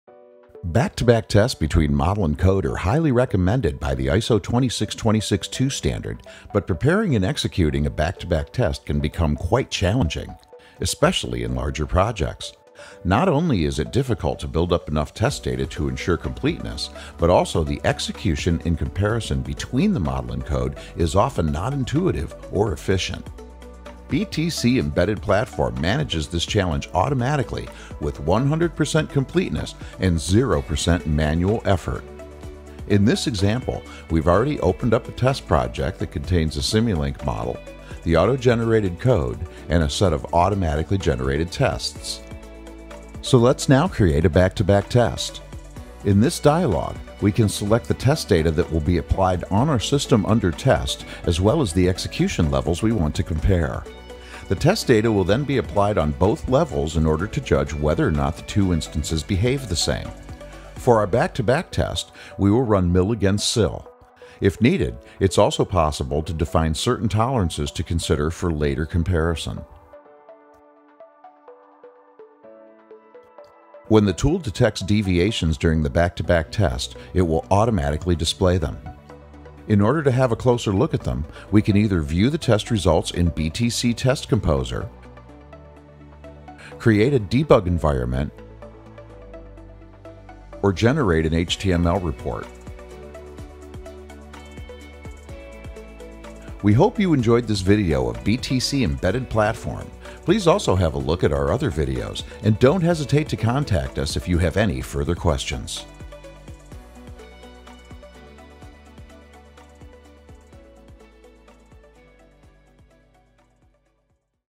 Back-to-Back Test
TC EmbeddedPlatform/BTC EmbeddedTester provides an ISO 26262 certified and automated Back-to-Back Test between models and production code for dSPACE TargetLink and Mathworks Embedded Coder.

This video shows you how to:

    - Select the desired tests and execution modes
    - Define tolerances
    - Run the Back-to-Back Test
    - Inspect results